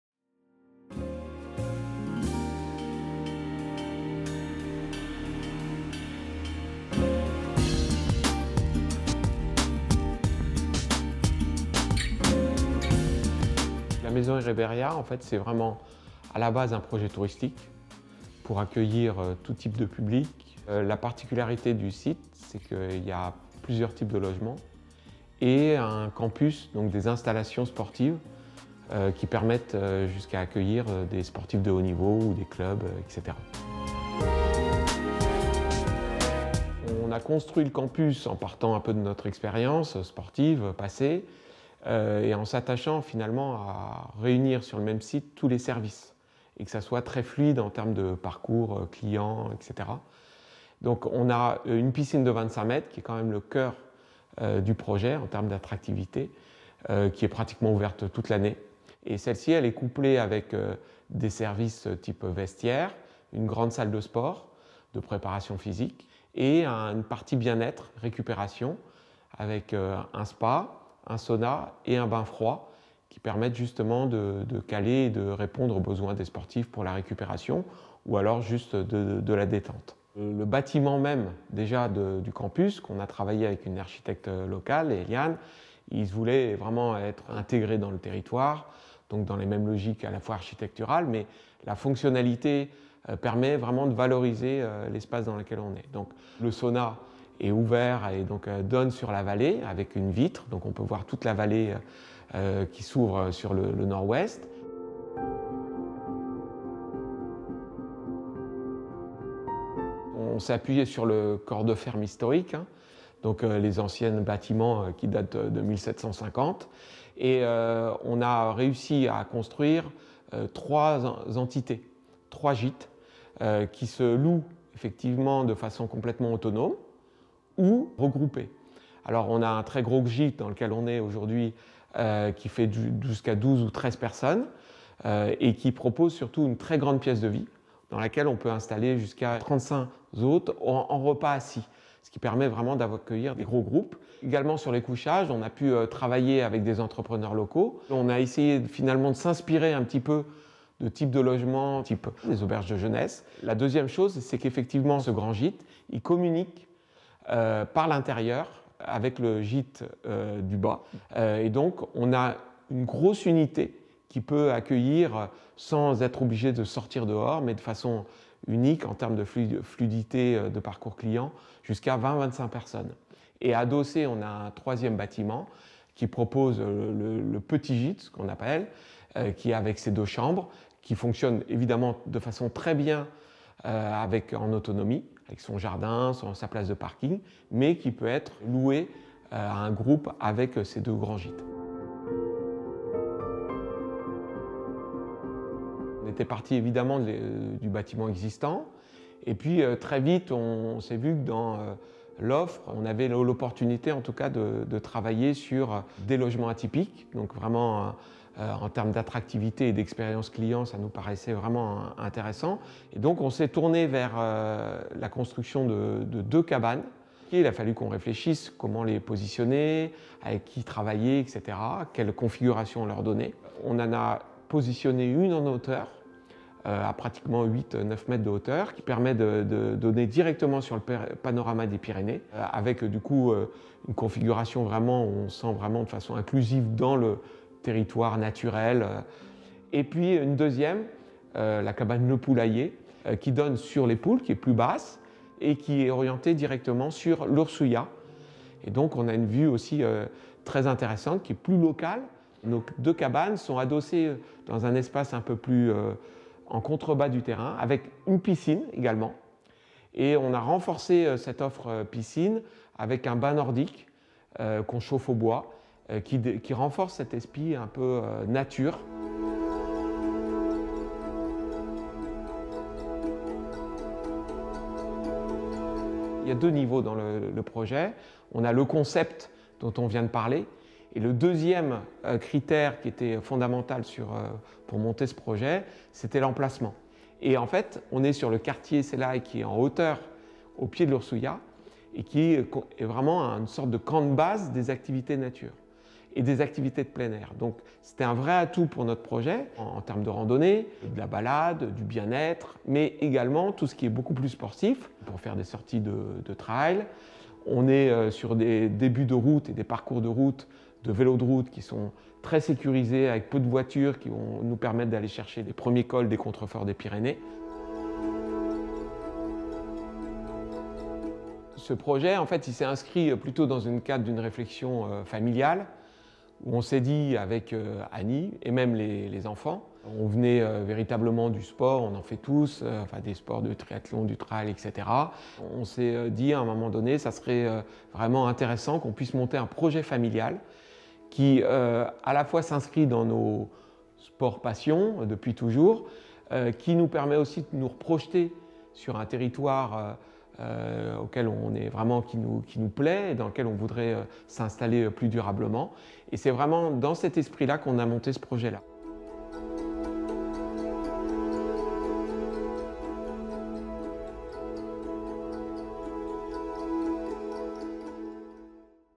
Maison Irriberria à Hasparren, lauréate des Trophées du tourisme 64, édition 2023
Voici notre lauréat de la catégorie EXPERIENCES VACANCES.
La maison Irriberria est un hébergement situé à Hasparren. Il propose gîte, cabane et campus sportif, pour les amateurs ou professionnels.
Les Trophées du tourisme valorisent les initiatives touristiques en Béarn Pays basque.